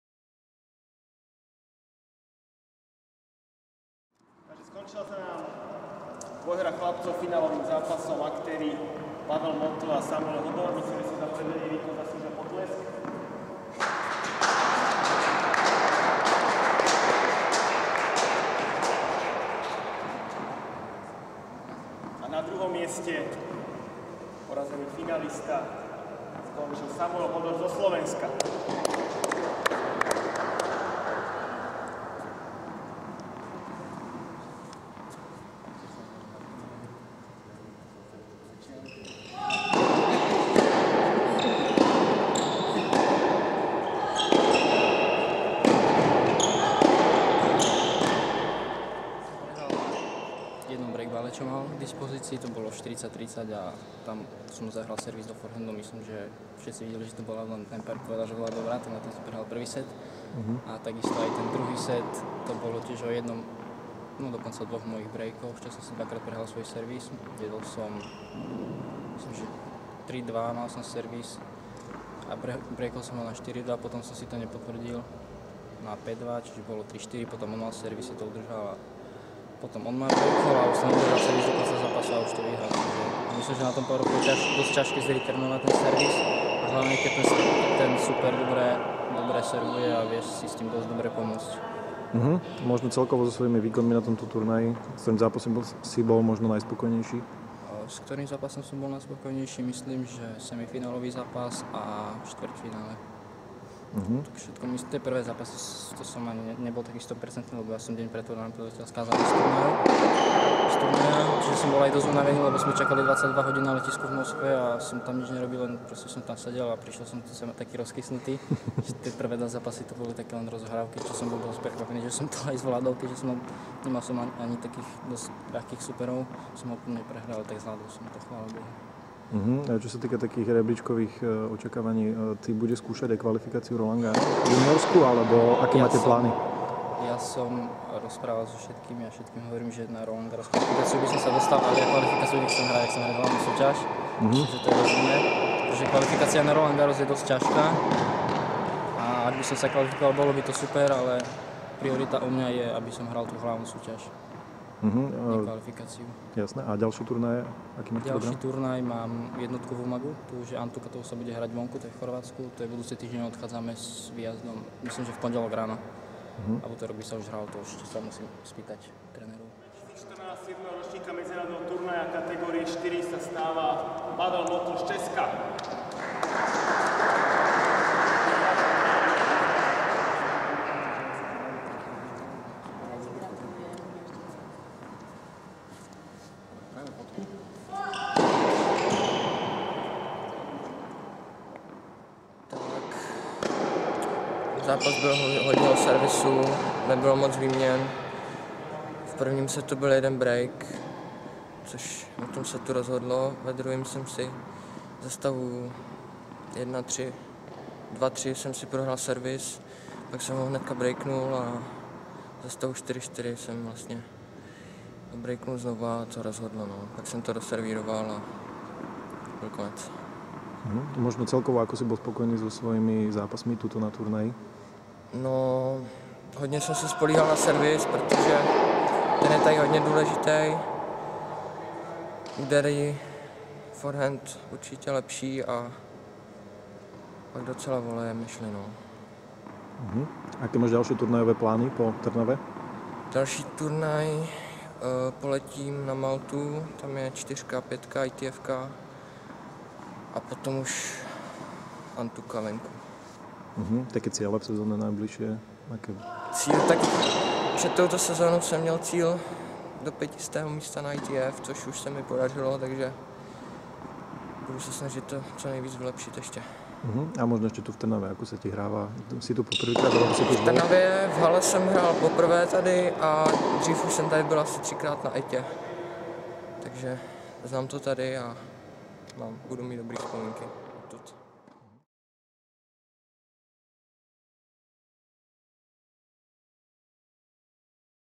TC EMPIRE TRNAVA: ITF U18 Empire Cup 2014, Ceremony & Interviews, Motl (CZE) - Hodor (SVK) 6-3,6-4
TC EMPIRE TRNAVA: ITF U18 Empire Cup 2014, Ceremony & Interviews after the Final, Pavel Motl (CZE) - Samuel Hodor (SVK) 6-3,6-4.